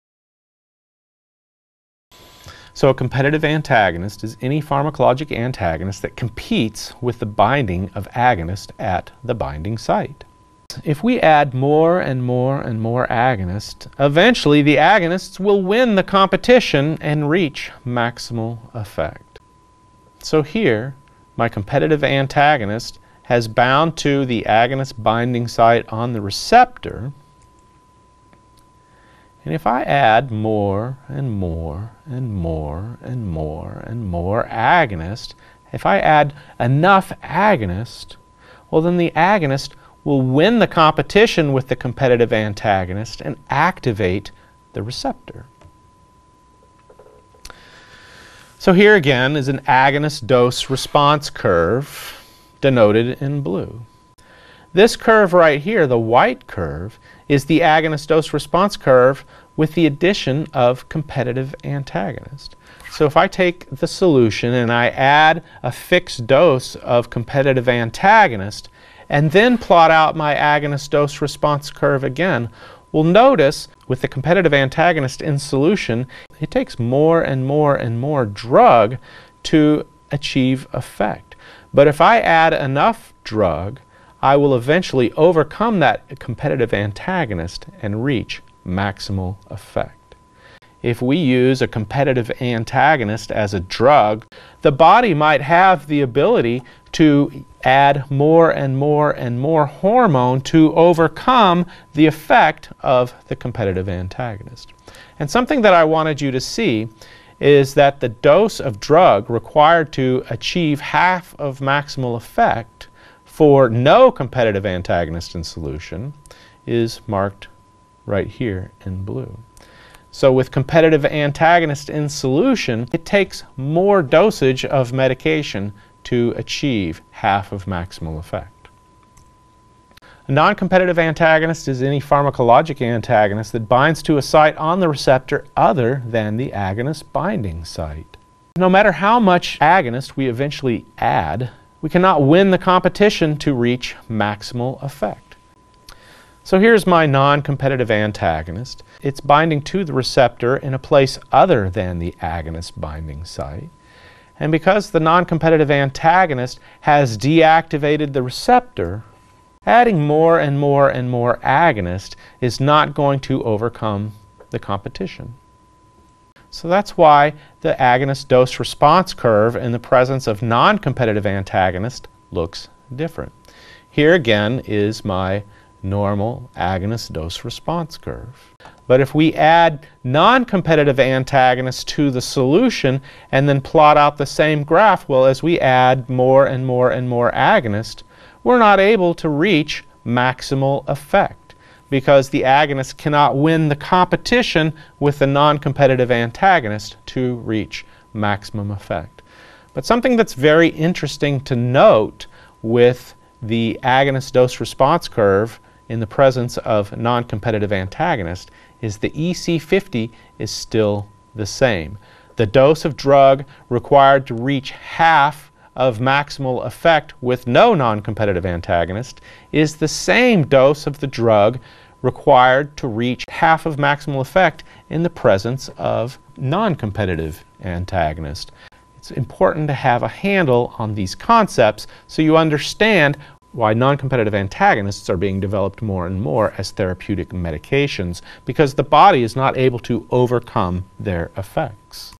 Agonist DR Curves with Competitive and Noncompetitive Antagonist
A description of Agonist Dose-Response Curves in the presence of Competitive Antagonist and Noncompetitive Antagonist.